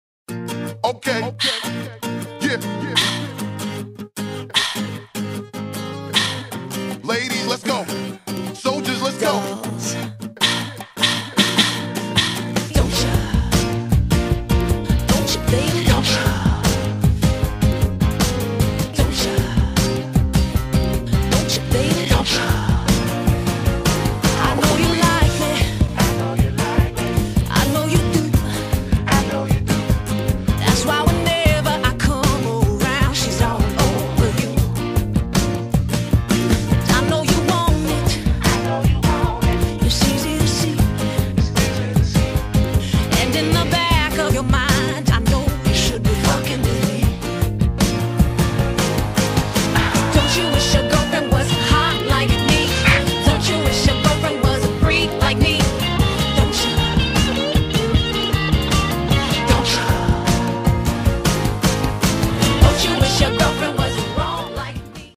Gameplay 06 - De Boothbabes
De eerste video van de Gameplay 06 van eigen hand. Ik begin makkelijk: alle plaatjes van de boothbabes achter elkaar en een aardig muziekje eronder.